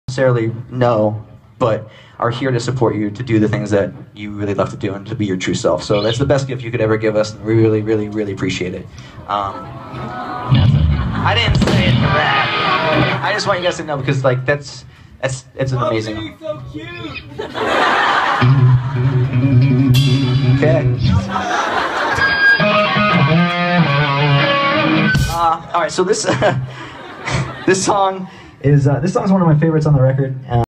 Someone tells Frank Iero to 'stop being cute' Barfly 8/11/14
I filmed this on my iPhone with no zoom, he was literally that close to me !! Barfly, Camden for Frnkiero Andthe Cellabration on the 8th of November 2014